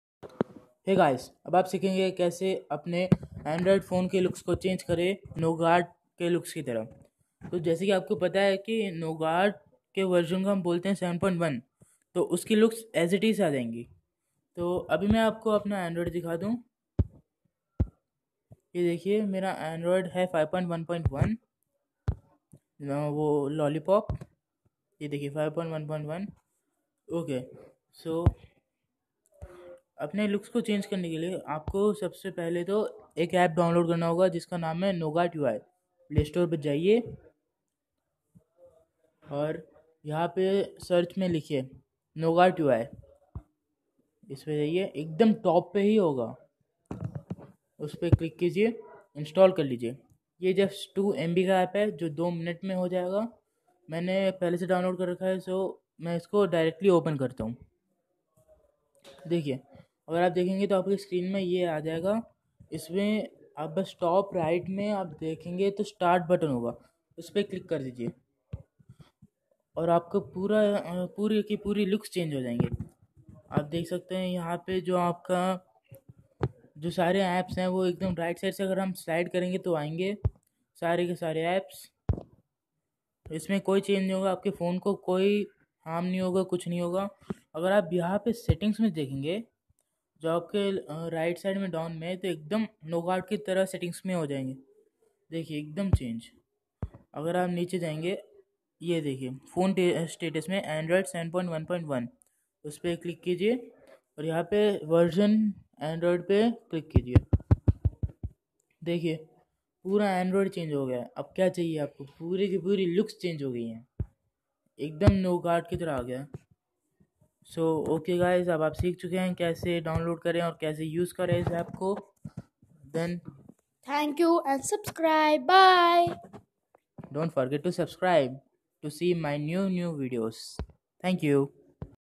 How to change your Android to nougat no root just in 3 MB
Hey guys! Thank you for seeing.
First you have to go to play store and download an app named nougat ui and then​ start.